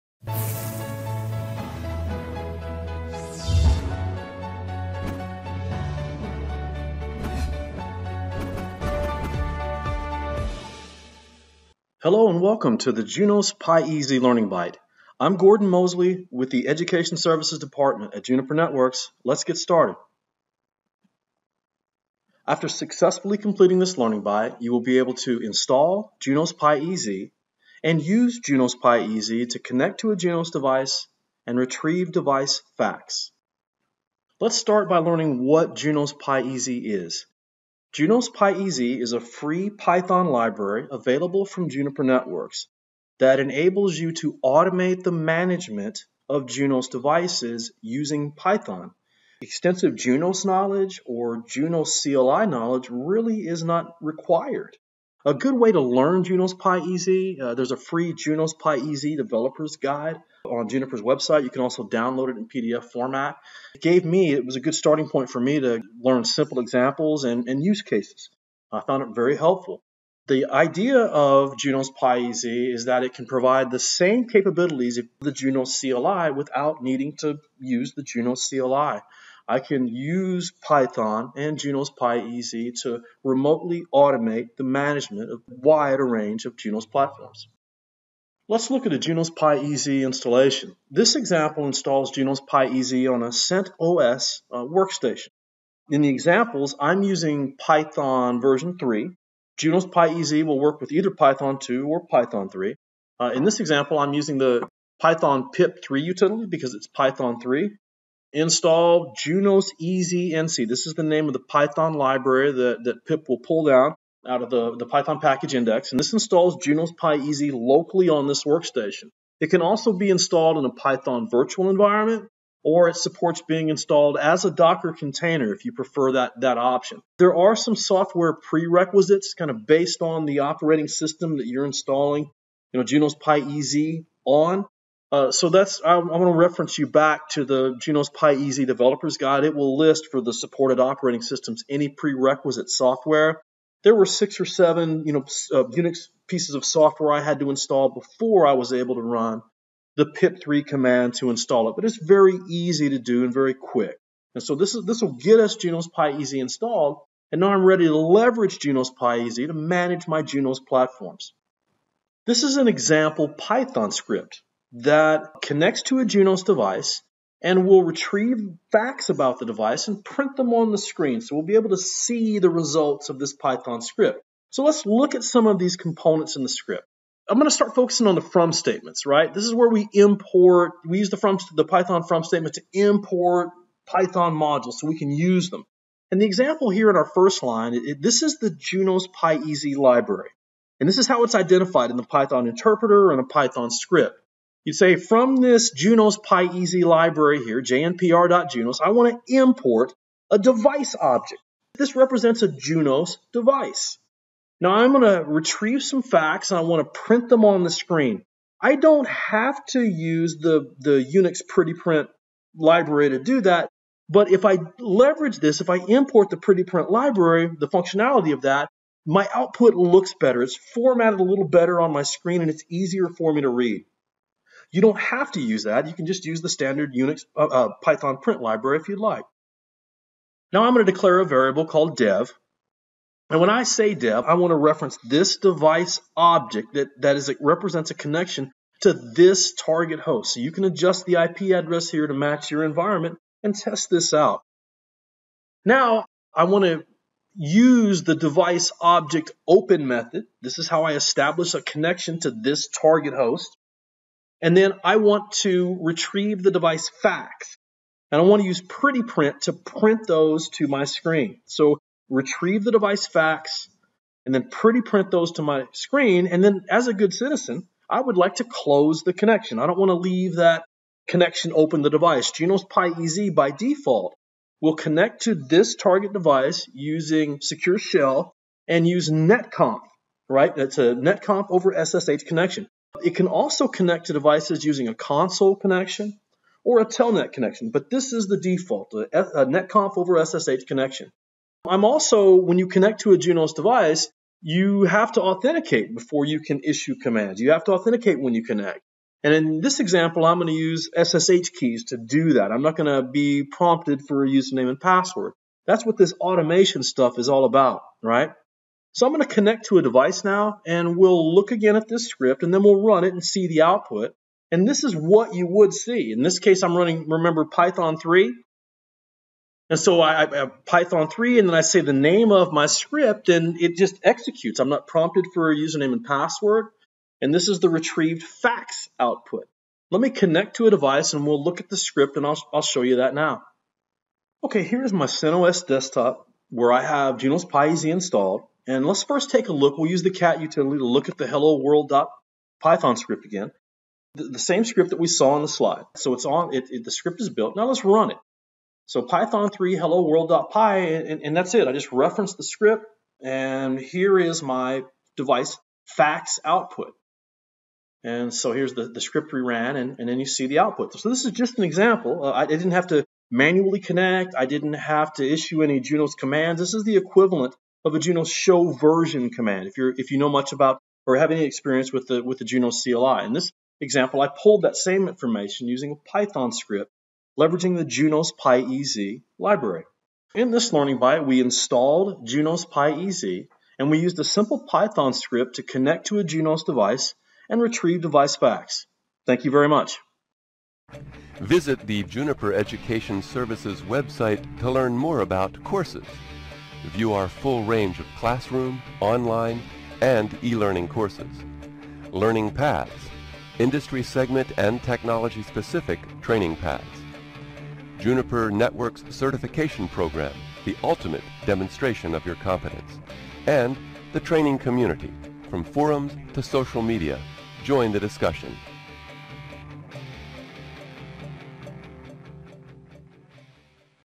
Junos PyEZ
This video demonstrates installing Junos PyEZ and using it to retrieve Junos device facts.
• Presenter: Gordon Mosley

• Relevant to Junos OS Releases: All

• Relevant to Juniper Platforms: All

Series: Learning Bytes